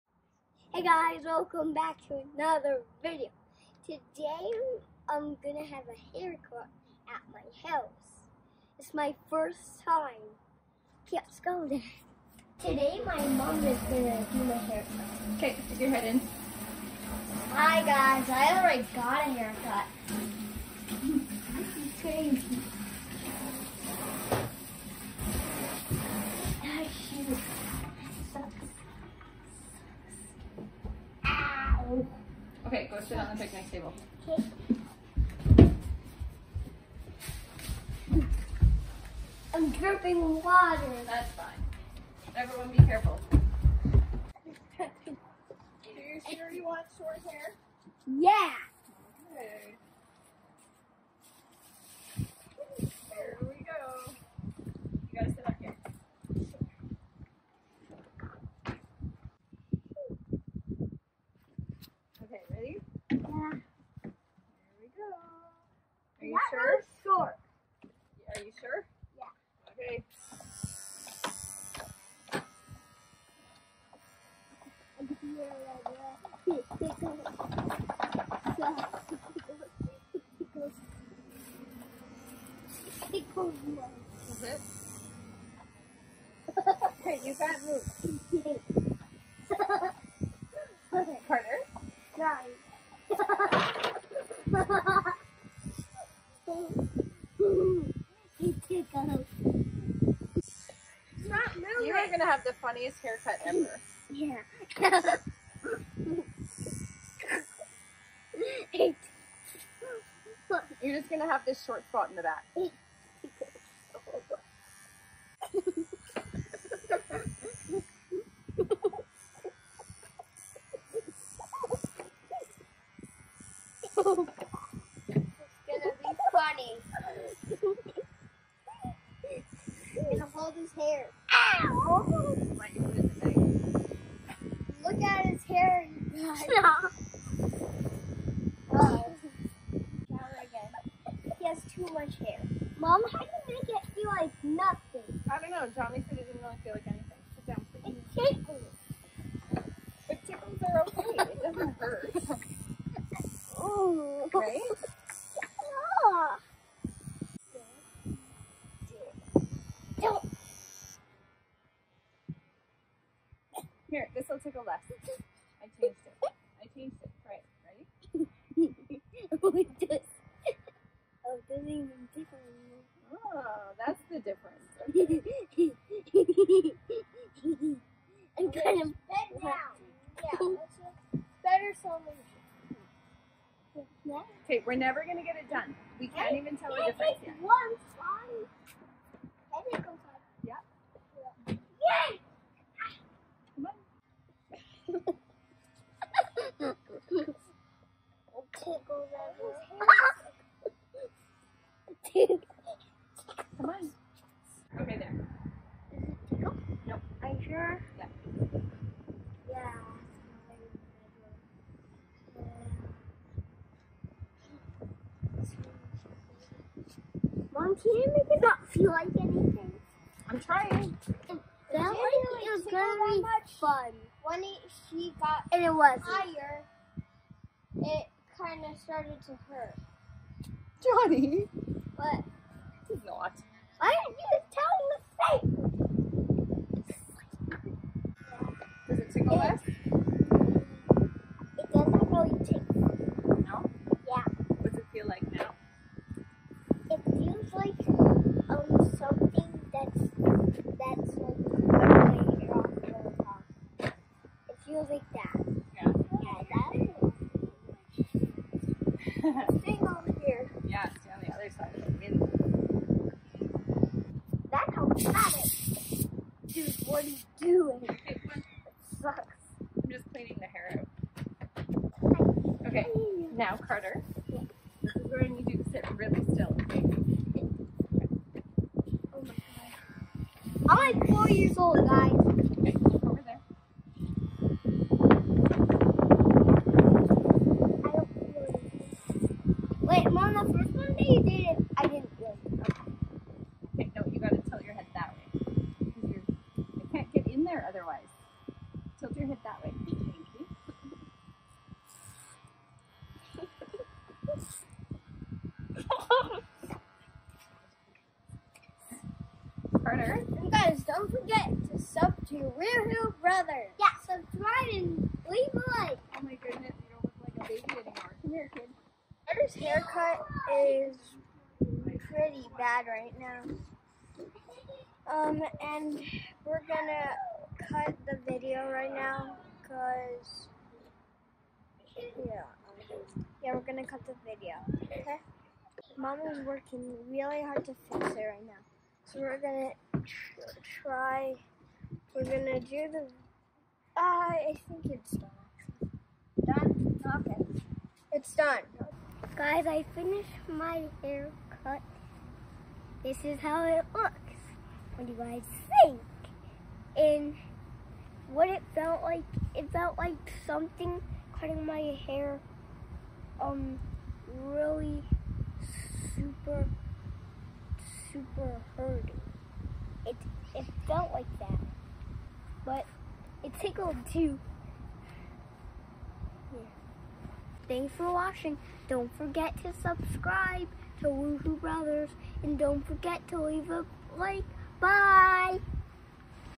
Carter Gets a Lockdown Haircut!
Carter's first haircut by mom while we're socially distancing from home!